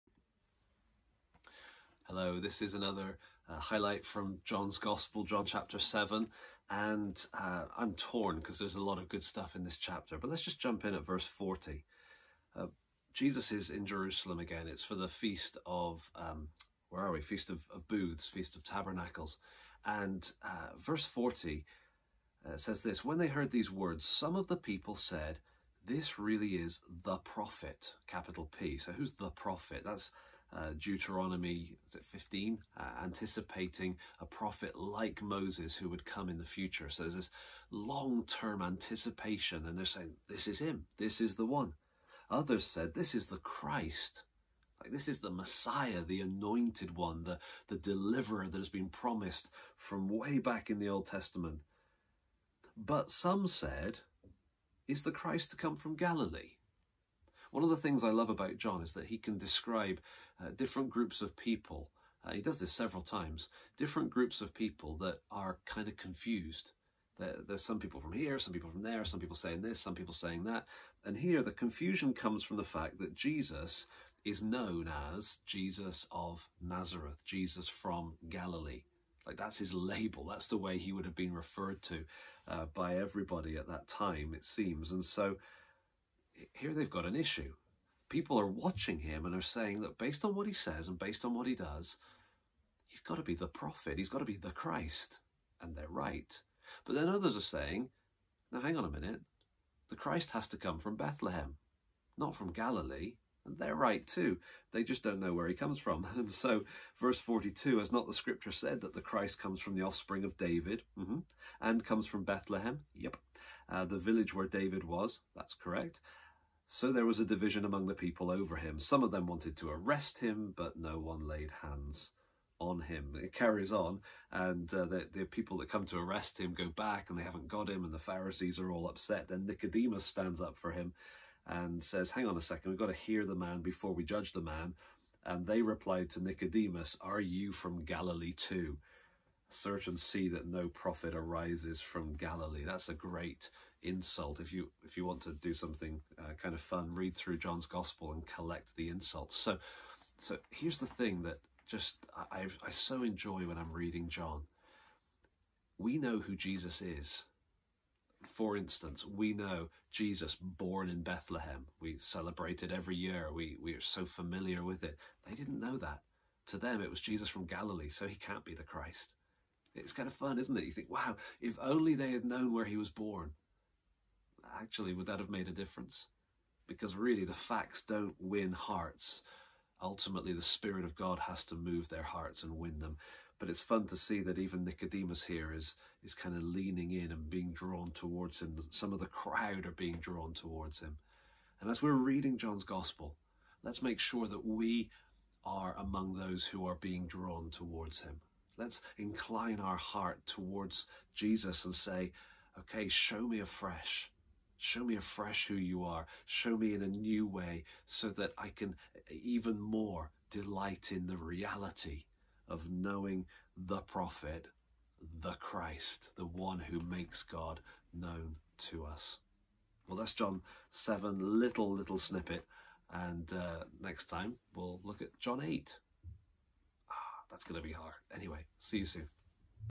Not From Galilee! (Pursuing God’s Heart - John 7:40ff)
Jesus of Nazareth was the name they used, but did it hide something important about him?

Pursuing God’s Heart are simple, biblical and relevant Bible highlights.  These are not polished presentations, crafted sermons or edited videos.